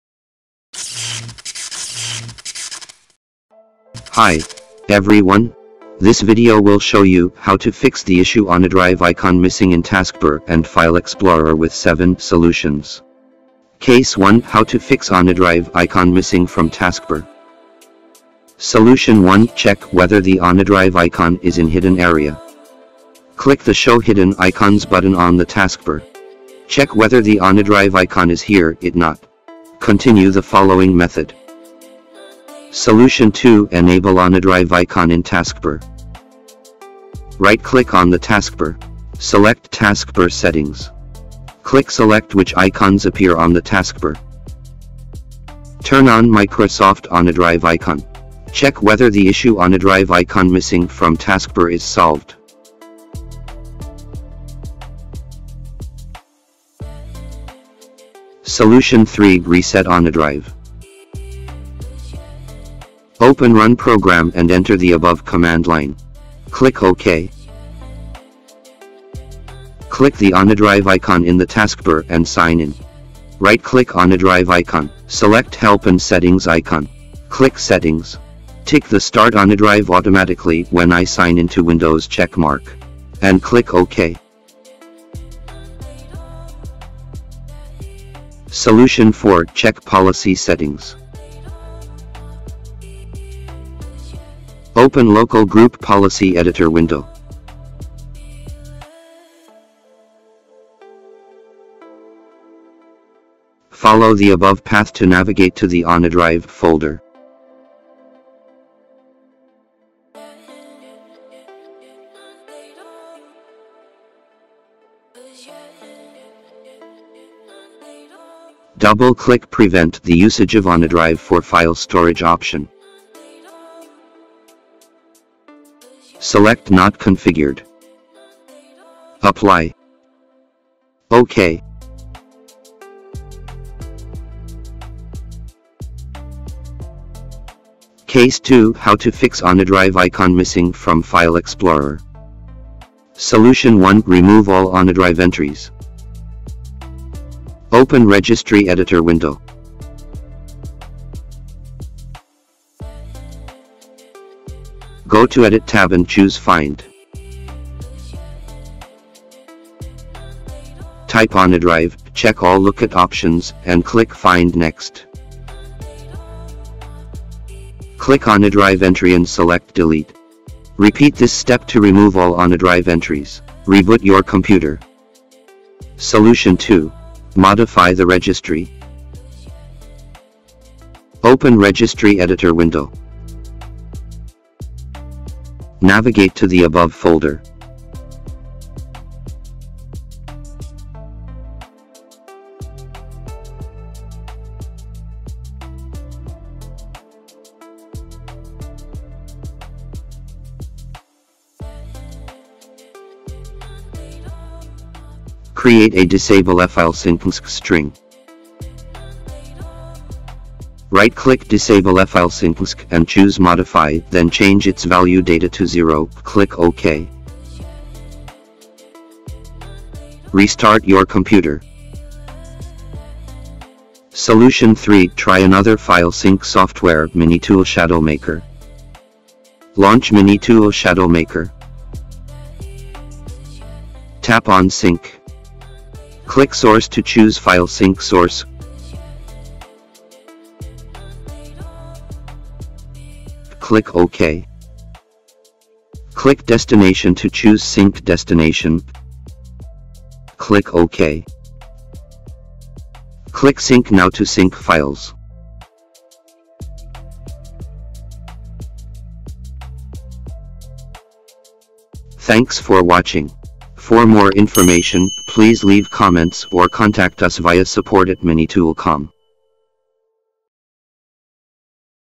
7 Solutions to OneDrive Icon Missing from Taskbar and File Explorer
OneDrive is a file hosting service and synchronization service. It helps users store and protect your files, and share them with others. But sometimes, you’ll find its icon is missing from Taskbar and File Explorer. How to solve the issue OneDrive icon missing? This video offers 7 solutions.
To know more information about how to fix the issue OneDrive icon missing in Taskbar and File Explorer, please read this post
Solution 1: Check Whether the OneDrive Icon Is in Hidden Area    0:19-0:30
Solution 2: Enable OneDrive Icon in Taskbar    0:32-0:56
Solution 3: Reset OneDrive   0:58-1:27
Solution 4: Check Policy Settings   1:28-2:15
Solution 5: Remove All OneDrive Entries   2:21-2:53
Solution 6. Modify the Registry   2:56-3:40
Solution 3: Try Another File Sync Software – MiniTool ShadowMaker   3:44-4:20